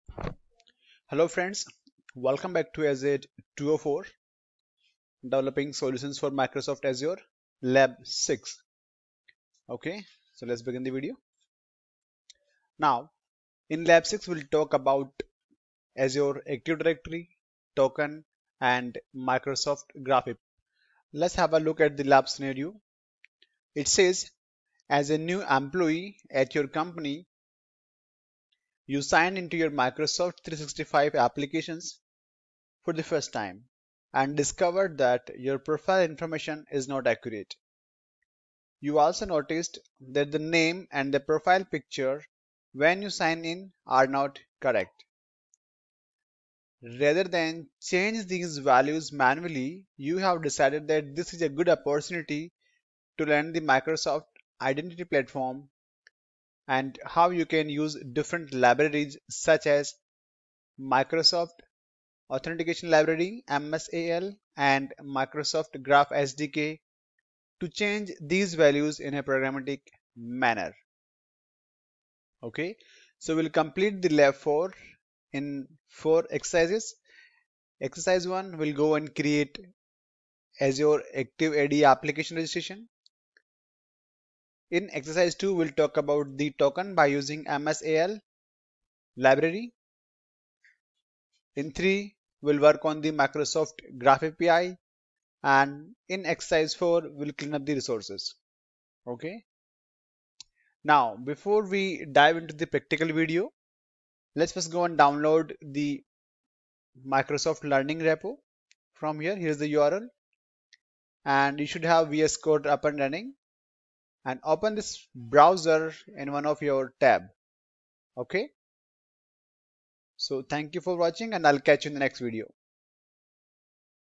AZURE | AZ 204 | Lab 06 | Introduction | okay java
AZ 204 Lab 06: Implement user authentication and authorization
Introduction : Authenticating to and querying Microsoft Graph by using MSAL and .NET SDKs
Exercise 01 : Create an Azure AD application registration
Exercise 02 : Obtain a token by using the MSAL.NET library
Exercise 03 : Query Microsoft Graph by using the .NET SDK
Exercise 04 : Clean up your subscription
AZ-204: DEVELOPING SOLUTIONS FOR MICROSOFT AZURE